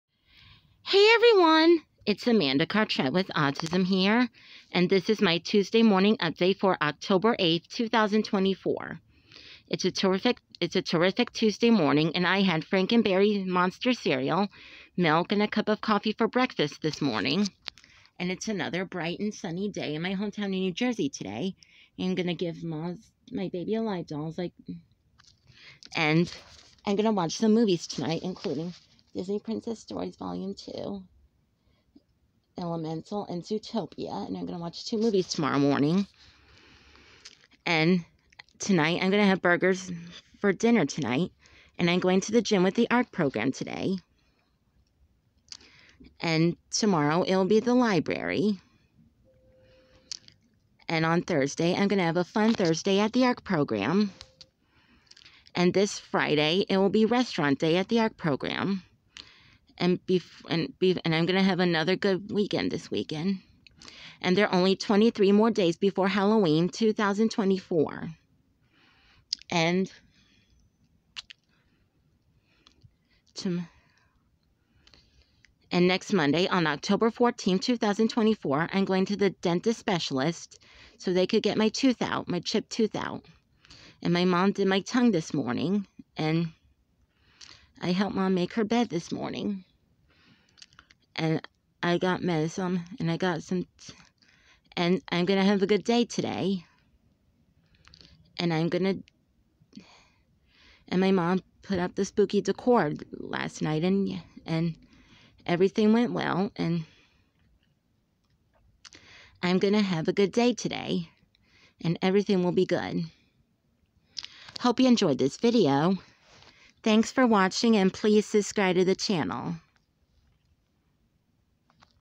Tuesday Morning Update For October 8, 2024
This Is My Tuesday Morning Update For October 8, 2024. It's A Terrific Tuesday Morning And I Had Franken Berry Monster Cereal, Milk, And A Cup Of Coffee For Breakfast This Morning, I Am Going To The Gym With The Arc Program Today, And I Am Going To Have Burgers For Dinner Tonight. Tomorrow It Will Be The Library, On Thursday I Am Going To Have A Fun Thursday At The Arc Program, And This Friday It Will Be Restaurant Day At The Arc Program. Note #1: There Are 23 More Days Before Halloween 2024. Note #2: I Am Going To Give Mom Some Baby Alive Dolls. Note #3: Next Monday On October 14, 2024 I Am Going To The Dentist Specialist To Get My Chipped Tooth Pulled Out. Note #4: I Am Going To Watch Some DVDs Tonight. Note #5: I Did Sleep Well Last Night.